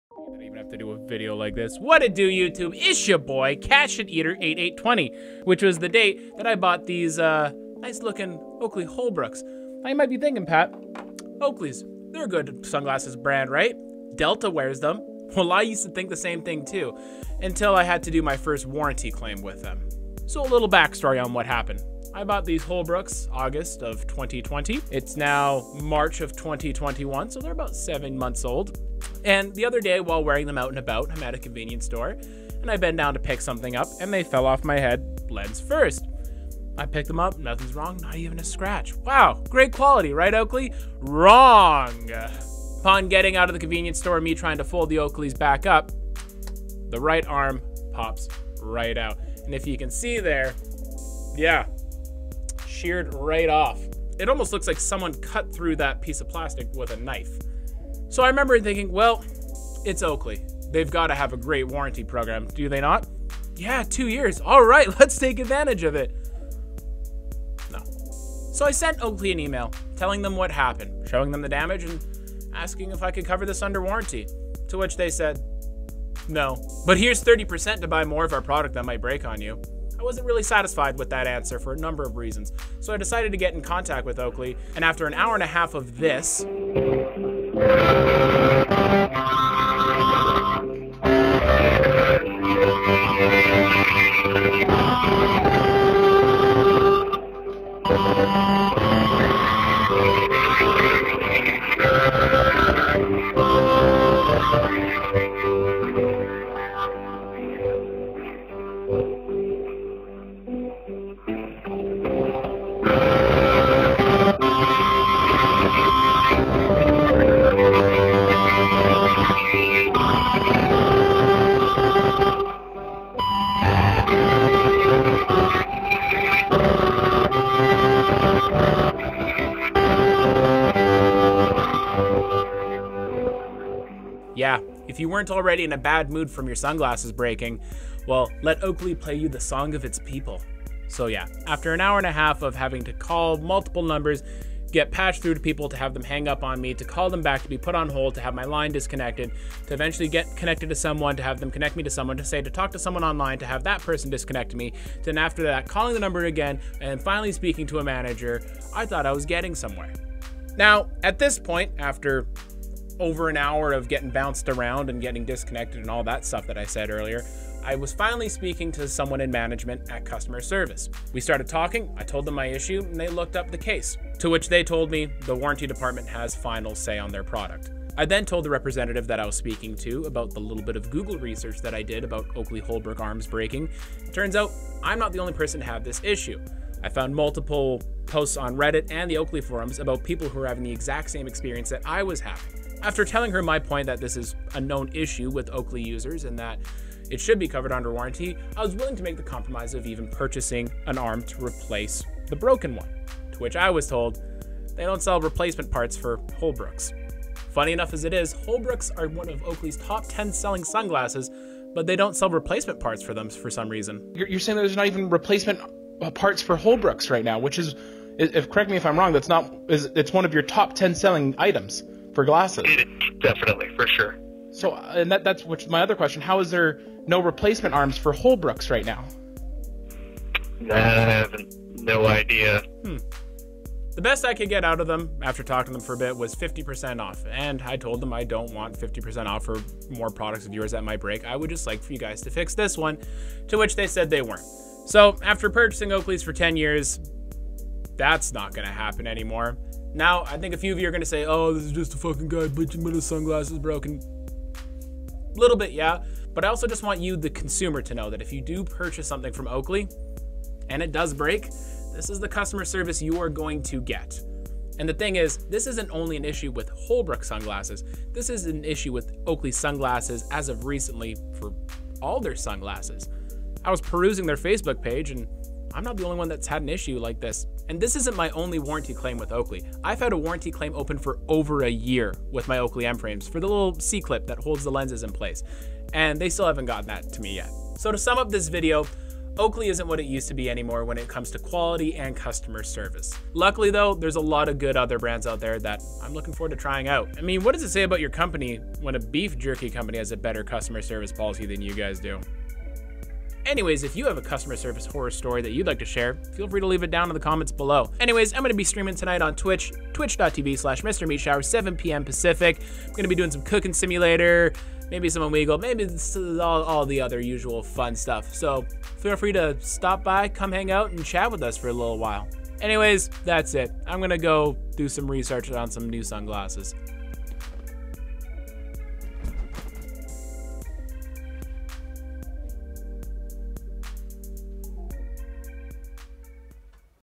When did Oakley Sunglasses Get Bad!?! (Warranty Horror Story)
Buyer beware! Oakley is not what it used to be. Cheap products and no customer service to back up their products anymore. Here is my experience while trying to get my Holbrooks fixed.

Streaming tonight on twitch!